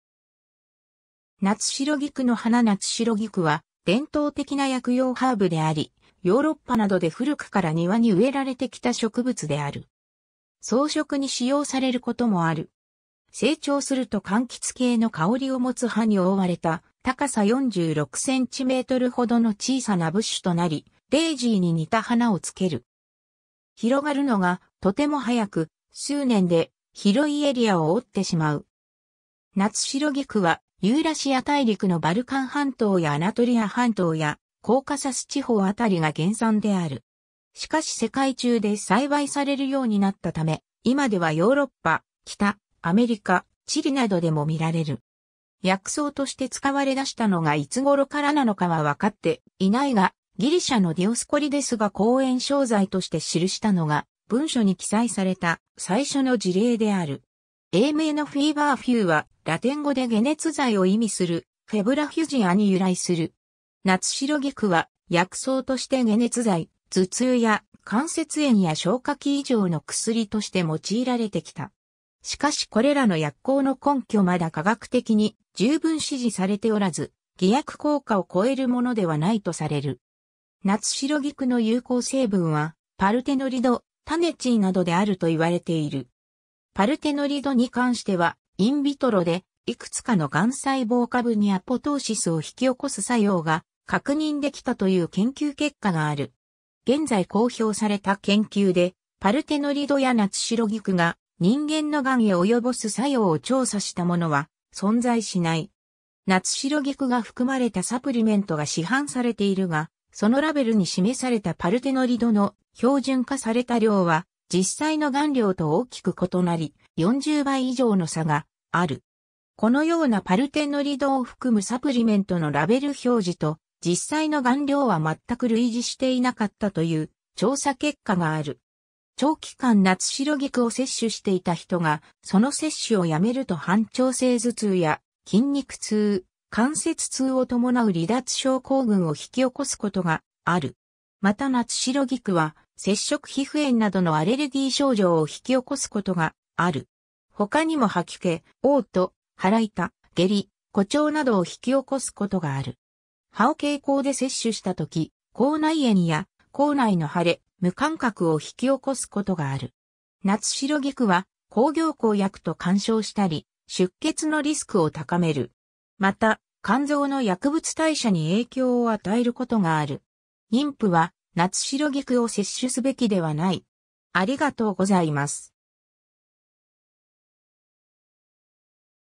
ナツシロギク
ナツシロギクの花 ナツシロギク (英名：feverfew;フィーバーフュー、学名： Tanacetum parthenium) は伝統的な薬用ハーブであり、ヨーロッパなどで古くから庭に植えられてきた植物である。
装飾に使用されることもある。
成長すると柑橘系の香りをもつ葉に覆われた高さ46センチメートルほどの小さなブッシュとなり、デイジーに似た花をつける。
広がるのがとても早く、数年で広いエリアを覆ってしまう。
 ナツシロギクはユーラシア大陸のバルカン半島やアナトリア半島やコーカサス地方辺りが原産である。
しかし世界中で栽培されるようになったため、今ではヨーロッパ、北アメリカ、チリなどでも見られる。
 薬草として使われだしたのがいつ頃からなのかは分かっていないが、ギリシャのディオスコリデスが抗炎症剤として記したのが、文書に記載された最初の事例である。
 英名のfeverfewはラテン語で解熱剤を意味するfebrifugiaに由来する。
ナツシロギクは薬草として解熱剤、頭痛や関節炎や消化器異常の薬として用いられてきた。
しかしこれらの薬効の根拠まだ科学的に十分支持されておらず、偽薬効果を超えるものではないとされる。
 ナツシロギクの有効成分はパルテノリド、タネチンなどであると言われている。
パルテノリドに関しては、インビトロでいくつかの癌細胞株にアポトーシスを引き起こす作用が確認できたという研究結果がある。
現在公表された研究で、パルテノリドやナツシロギクが人間のガンへ及ぼす作用を調査したものは存在しない。
ナツシロギクが含まれたサプリメントが市販されているが、そのラベルに示されたパルテノリドの「標準化」された量は、実際の含量と大きく異なり、40倍以上の差がある。
このようなパルテノリドを含むサプリメントのラベル表示と実際の含量は全く類似していなかったという調査結果がある。
 長期間ナツシロギクを摂取していた人が、その摂取をやめると反跳性頭痛や筋肉痛、関節痛をともなう離脱症候群を引き起こすことがある。
またナツシロギクは接触皮膚炎などのアレルギー症状を引き起こすことがある。
他にも吐き気、嘔吐、腹痛、下痢、鼓腸などを引き起こすことがある。
葉を経口で摂取した時、口内炎や口内の腫れ、無感覚を引き起こすことがある。
ナツシロギクは抗凝固薬と干渉したり、出血のリスクを高める。
また肝臓の薬物代謝に影響をあたえることがある。
妊婦はナツシロギクを摂取すべきではない。